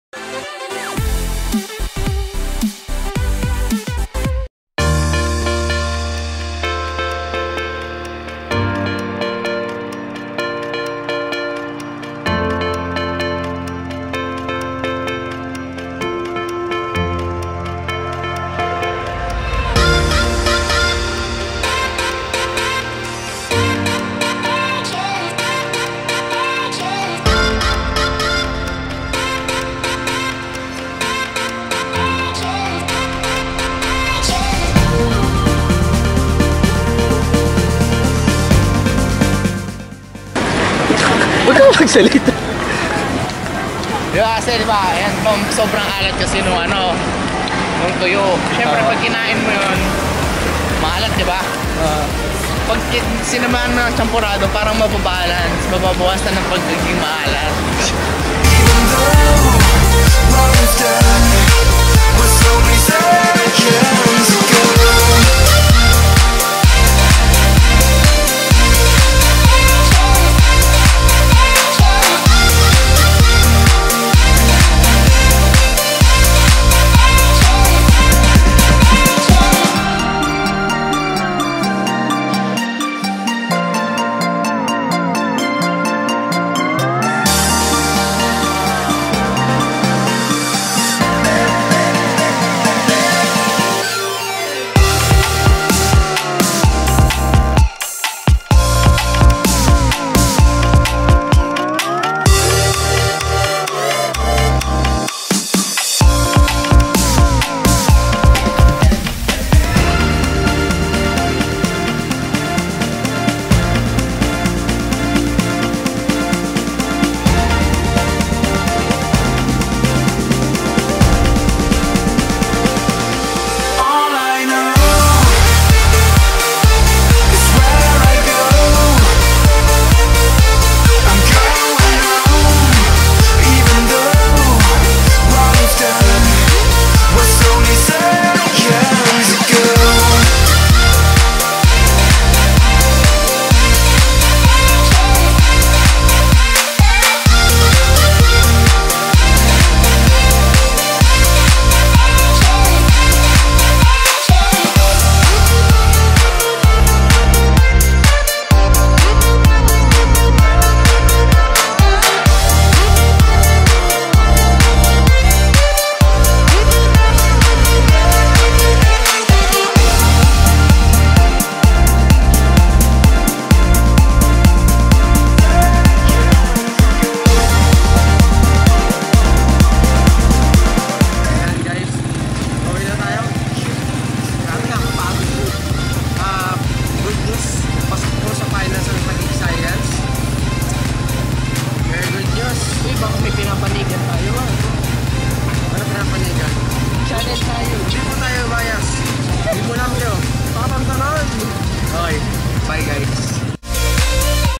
A Day at the NCC 2017
Solar Sports, ETC, and Jack TV sponsored the National Cheerleading Championship which was joined by different High Schools and Colleges to compete for the National Cheerleading Champion. The event was held at the SM Mall Of Asia Music Hall on January 29, 2017.